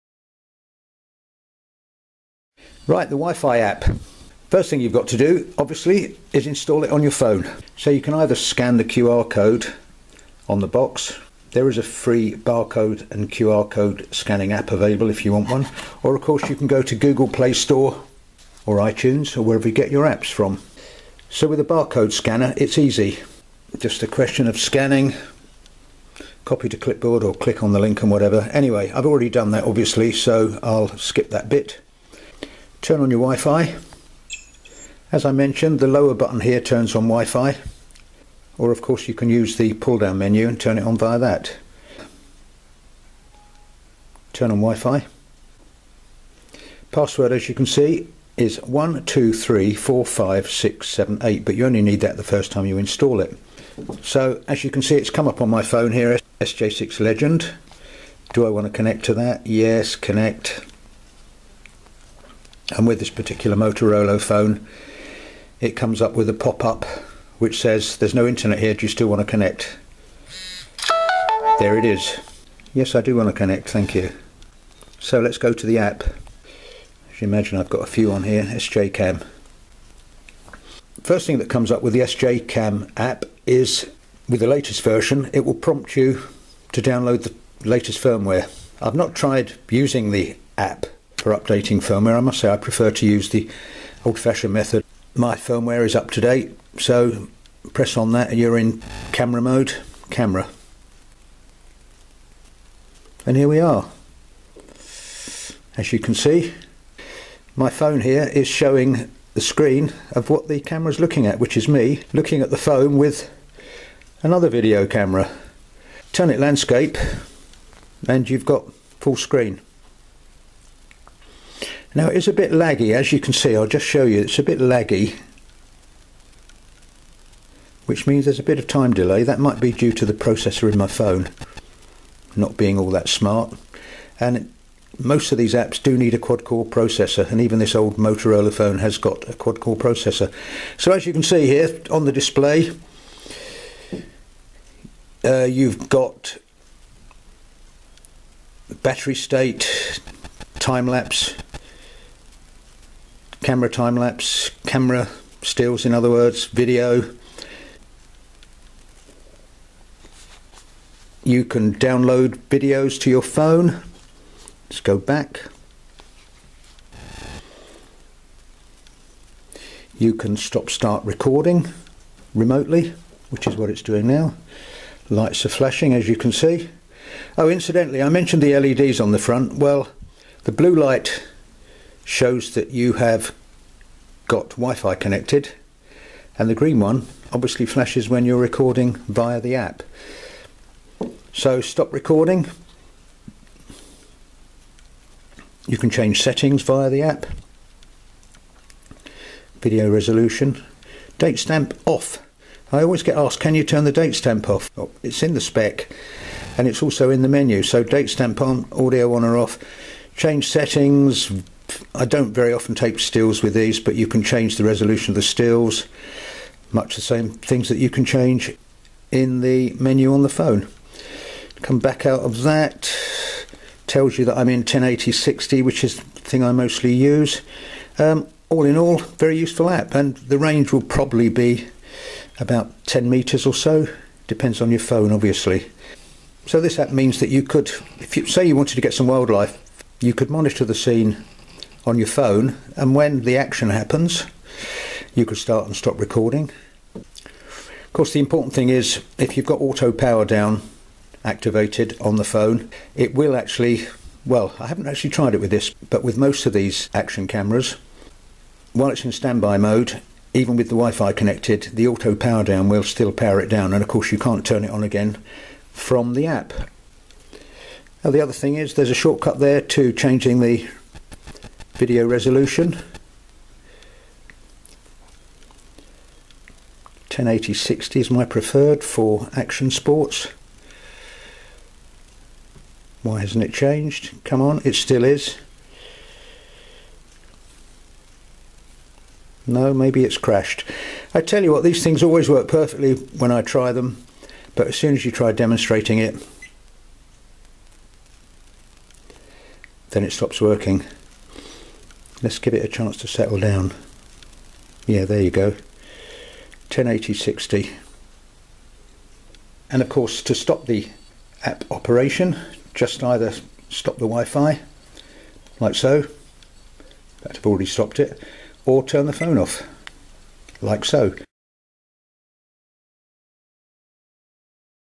SJCam WiFi app - Installing and Using - Action camera Wifi Apps Using
Step by step guide to installing the SJCam wifi app and using it.